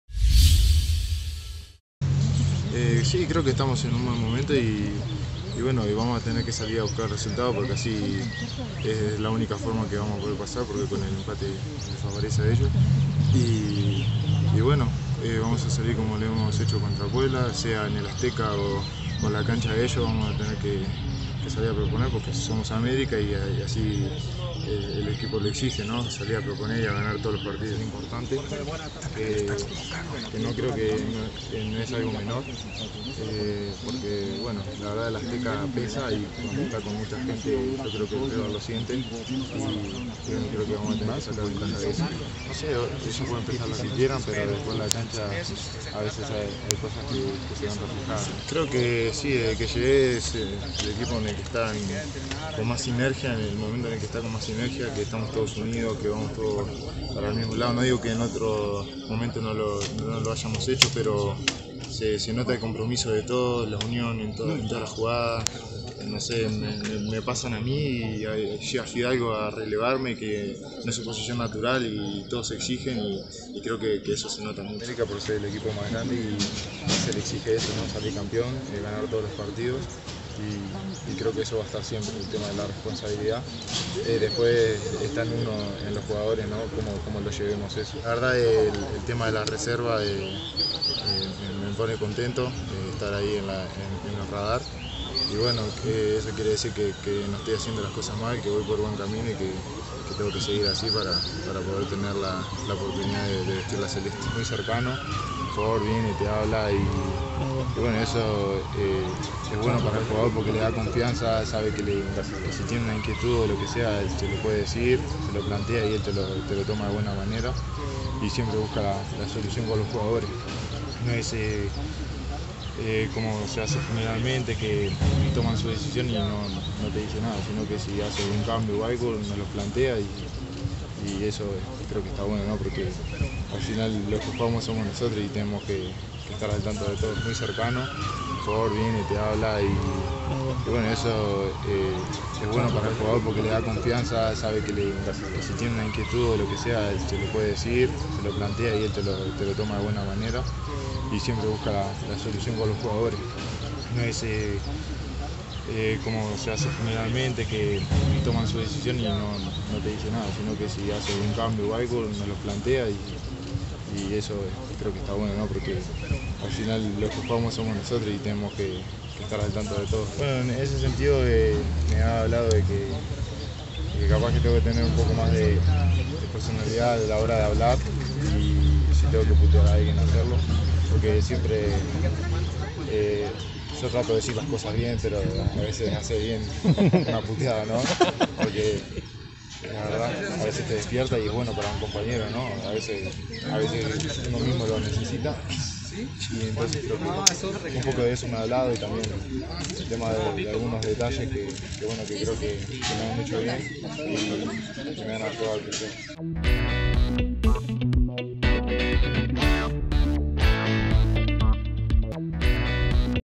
Sebastián Cáceres: Somos el equipo más grande y la exigencia es salir campeón
El zaguero de las Águilas del América destaca la unión y compromiso del equipo, de cara a la semifinal ante los Tuzos del Pachuca